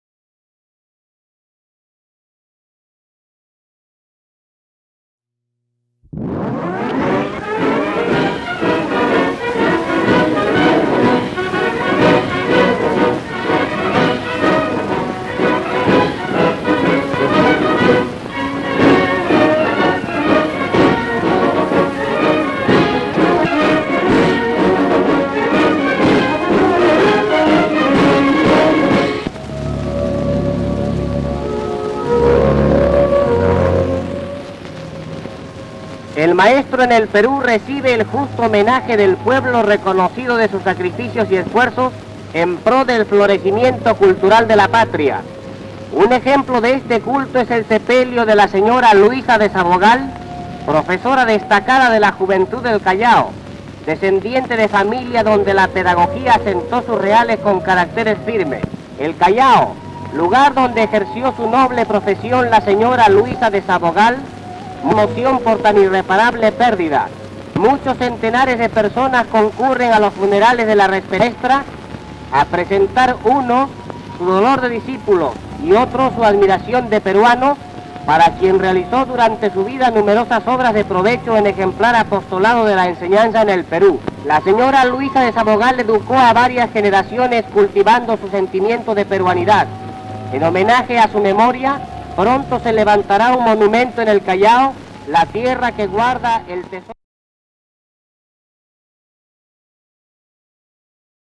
Homenaje a una maestra peruana
Imágenes del sepelio de Luisa de Sabogal, profesora del Callao, ejemplar trabajadora del sector educativo quien recibió una masiva despedida de parte de alumnos y público en general.

Noticiarios Peruanos de la década de 1940.
Película grabada en cintas de nitrato de 35 mm y digitalizada en la Biblioteca Digital - BNP.